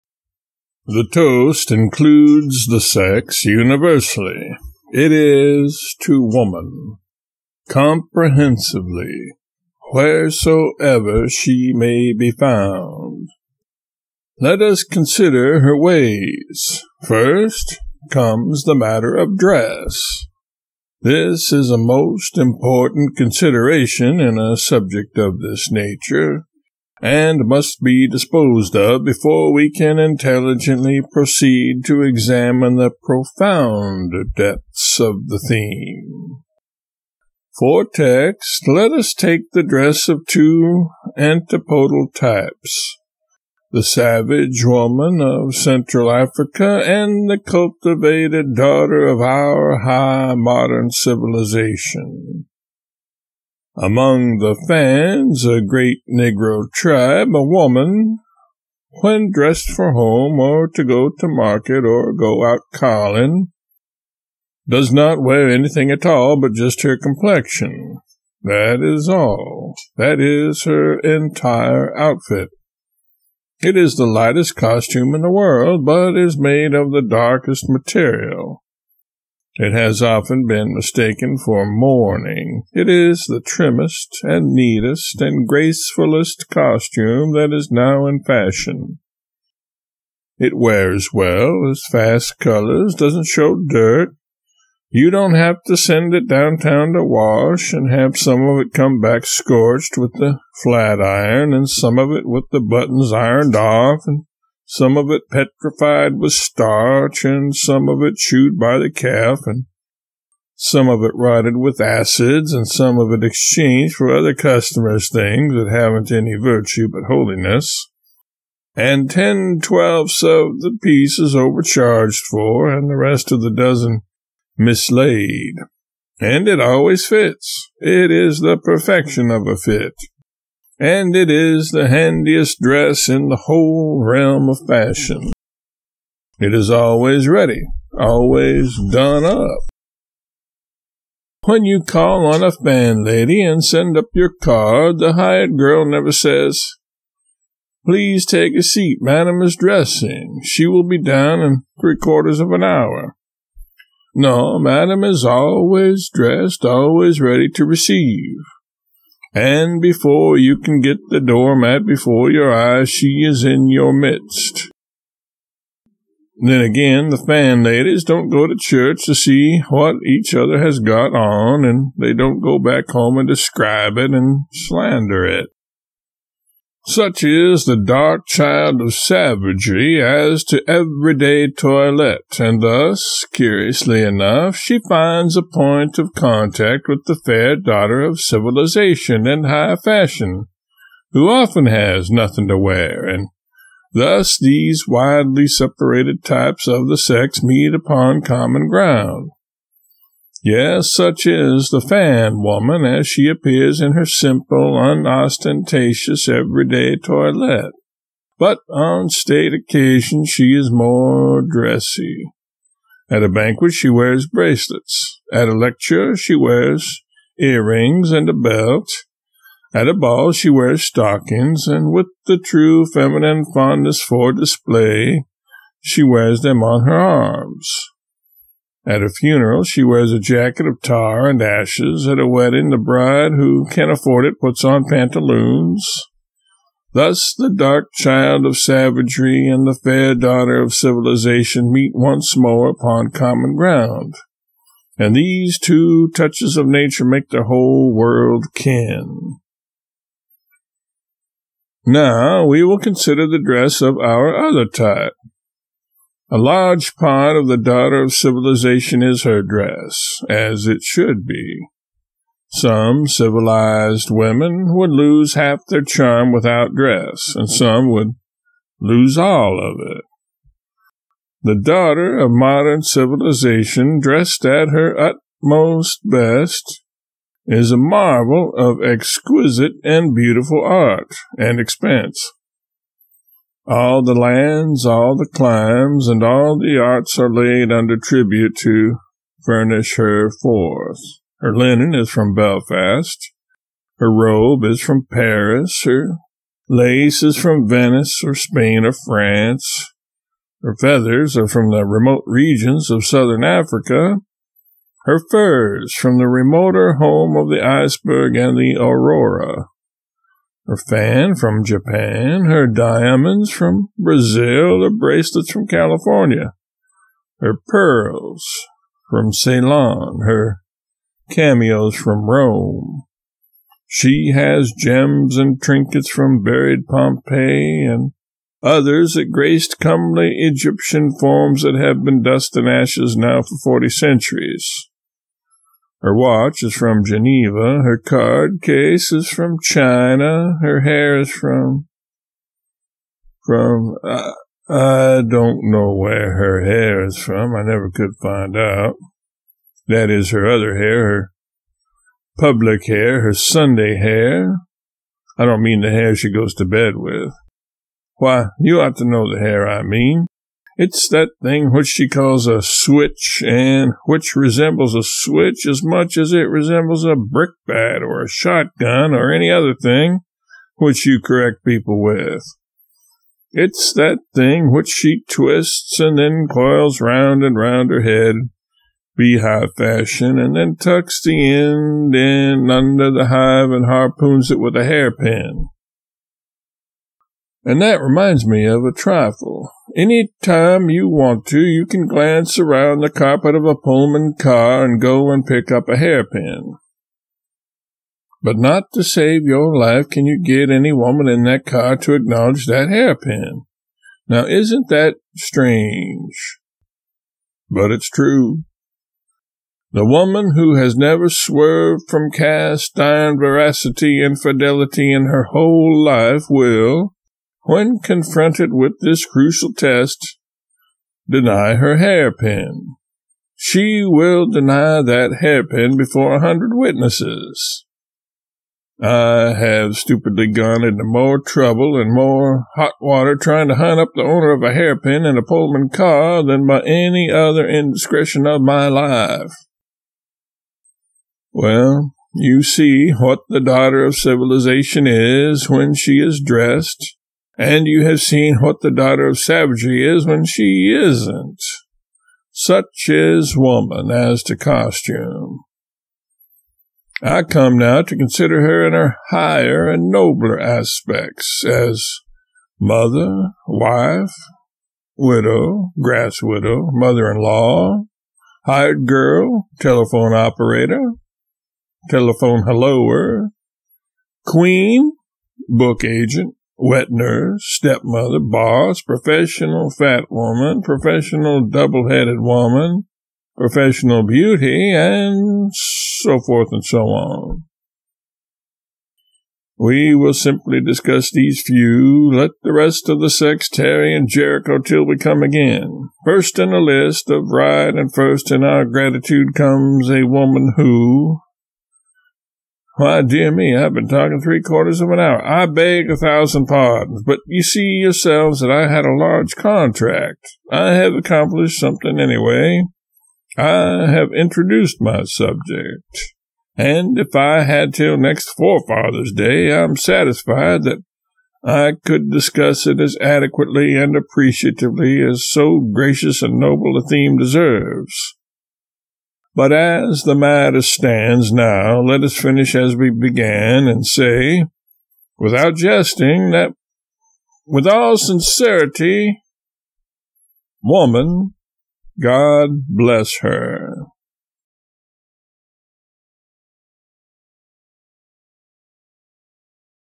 Mark Twain Woman God Bless Her Speech 1882
This is Mark Twain's Woman God Bless Her Speech 1882, read by Twain interpreter B. Clay Shannon